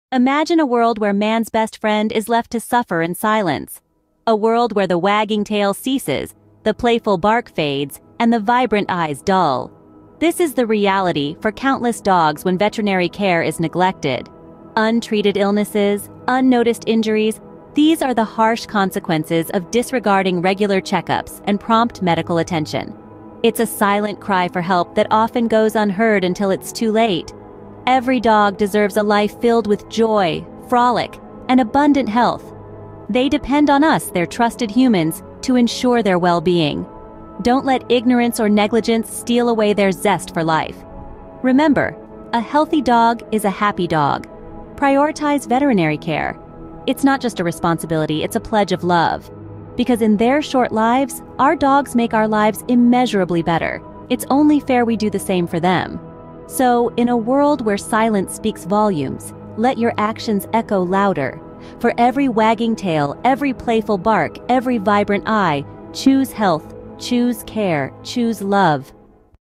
Unseen Pain The Silent Cry of Neglect
In this poignant video, we unveil the unseen pain and silent cry of neglect that plagues countless animals. Delve into the heart-wrenching reality of neglected animals, whose suffering often goes unnoticed and unheard. Through powerful imagery and heartfelt narration, we shed light on the urgent need for compassion and action to end the cycle of neglect and provide these voiceless creatures with the care and love they deserve. Join us in raising awareness and lending our voices to those who cannot speak for themselves. 🐾💔